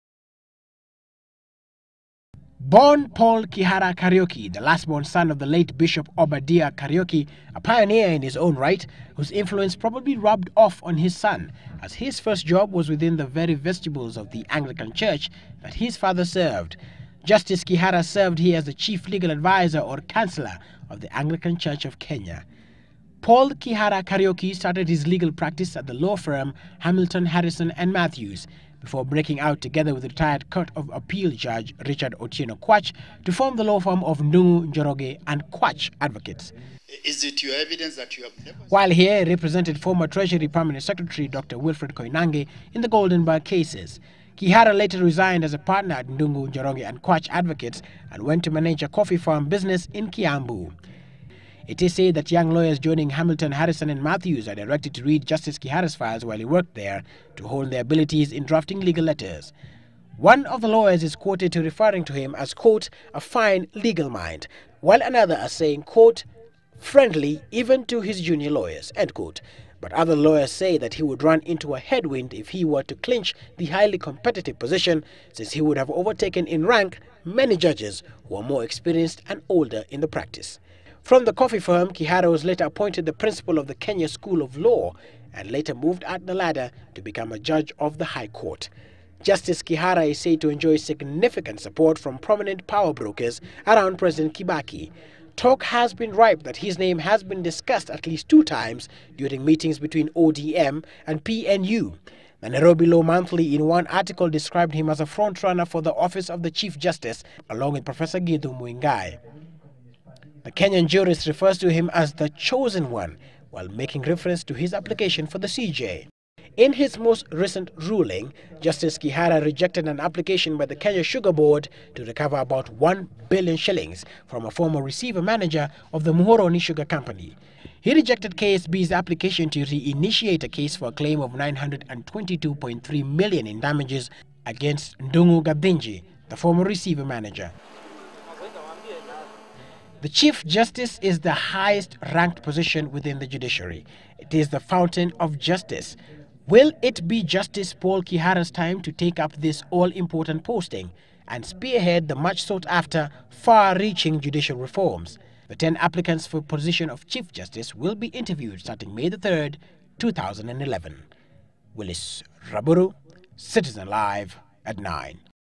Search for CJ/Justice Kihara
On our continuing series of profiles of candidates for Chief Justice, we focus on Justice Paul Kihara Kariuki. Now, he is the last born son of pioneer Anglican Bishop, the late Obadiah Kariuki, and is thought to enjoy support from powerbrokers around president Kibaki.